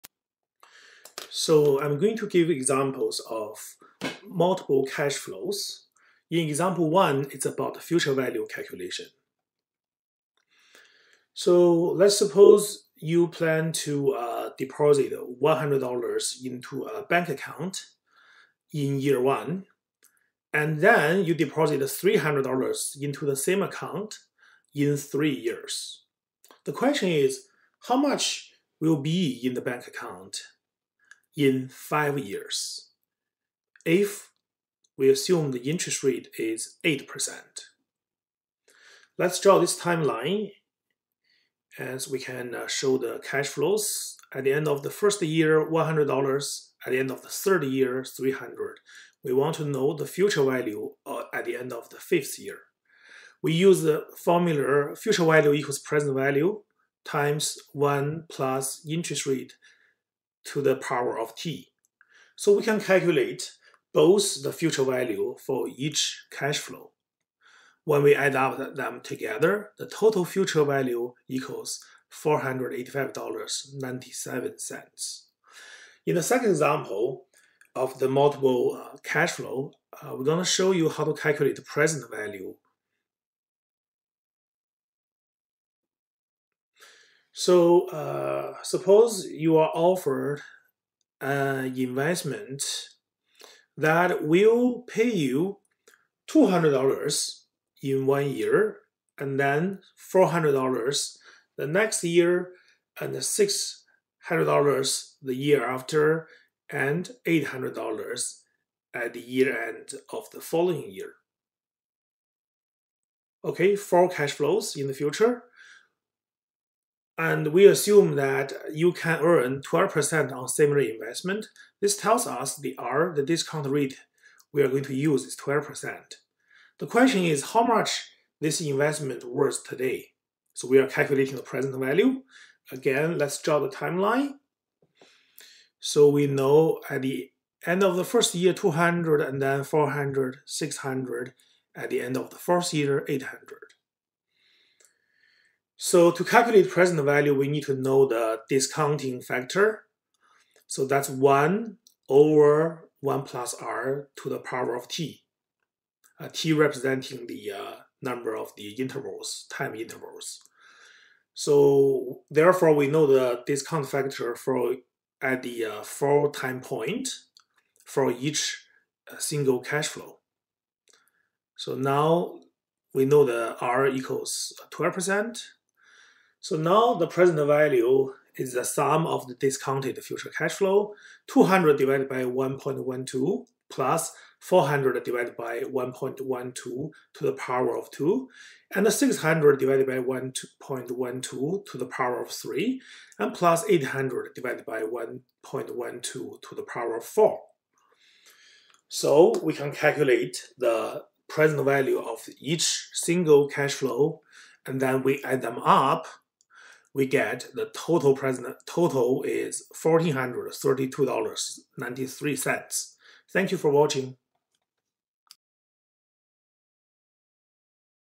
examples of multiple cash flow future value and present value calculations
The video explains the concepts of future value and present value calculations through two examples.
In the first example, it demonstrates how to calculate the future value of multiple cash flows. It describes a scenario where a person deposits $100 in Year One and $300 in Year Three, with an interest rate of 8%. By applying the future value formula, the total future value at the end of Year Five is found to be $485.97.
In the second example, the video explains how to calculate the present value of a series of cash flows. It outlines a scenario where an investment pays $200 in Year One, $400 in Year Two, $600 in Year Three, and $800 in Year Four, with a discount rate of 12%. Using the present value formula, the total present value of these cash flows is calculated to be $1,432.93.

Key terms:
multiple cash flows, future value, present value, discounting factor, example walk-through